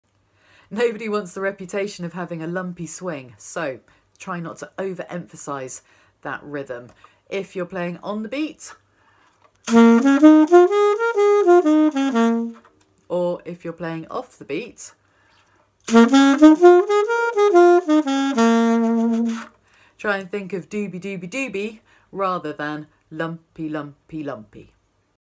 SOUNDING LUMPY? - swing quavers on and off the beat
Don’t try too hard…Swing quavers should flow or bounce but not be "lumpy".  This is a short demonstration of swing quavers both ON and OFF the beat